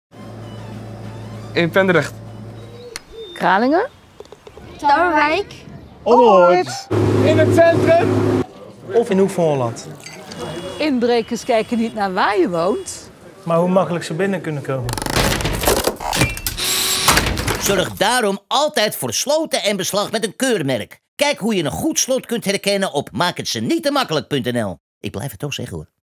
Woninginbraak Rotterdam voorkomen
Inbrekers kijken niet naar wáár je woont, maar hoe makkelijk ze kunnen binnenkomen. Met het Politiekeurmerk Veilig Wonen (PKVW) zorg je voor goede sloten op je woning. Kijk voor meer informatie op